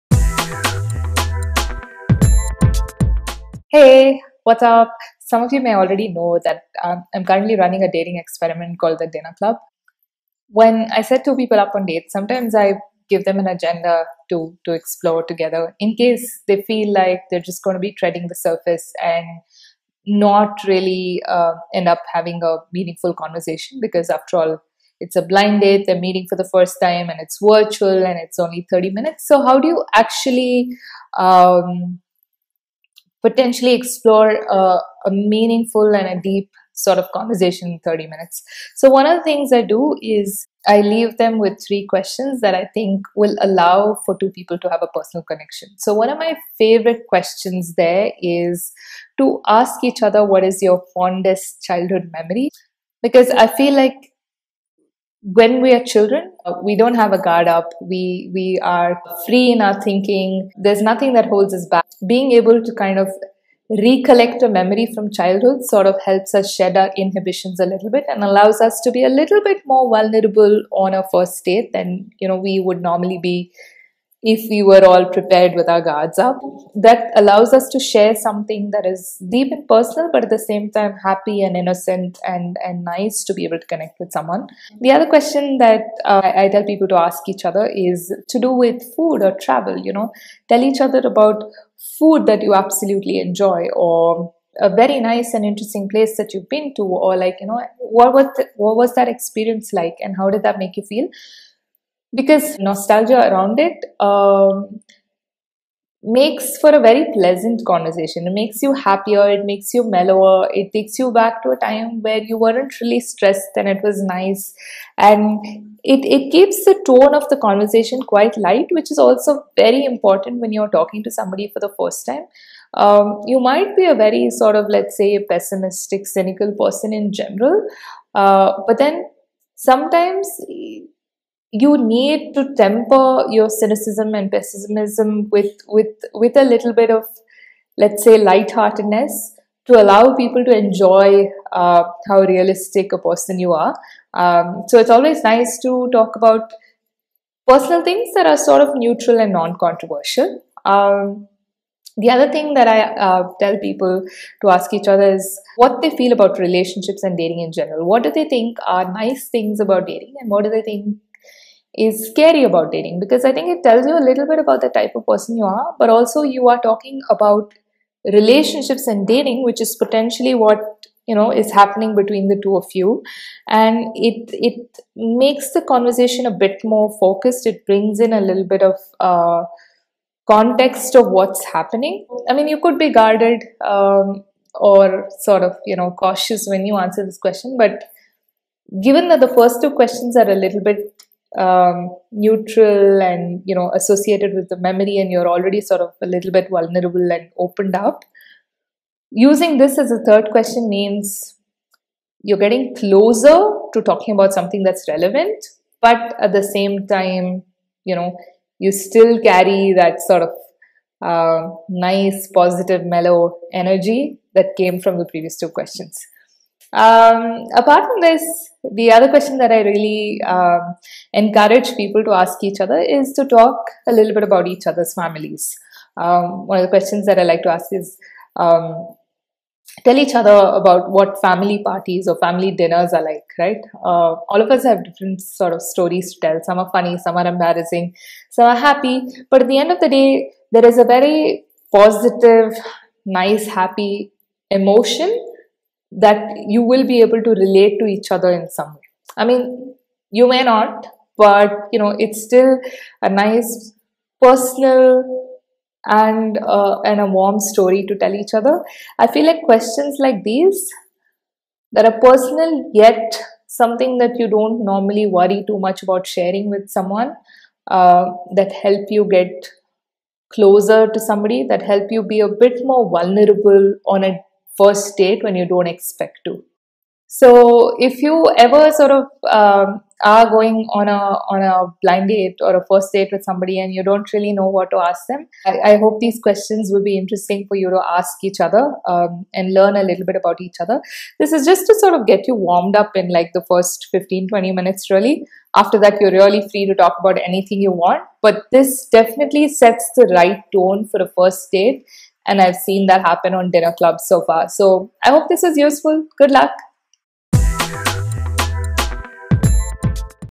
Simple ideas for a First Date Conversation
First dates are awkward by design. In this video, I talk about questions you can ask each other to build a deeper connection on a first date. While the questions may feel personal, they are not overly controversial or intrusive for a first date.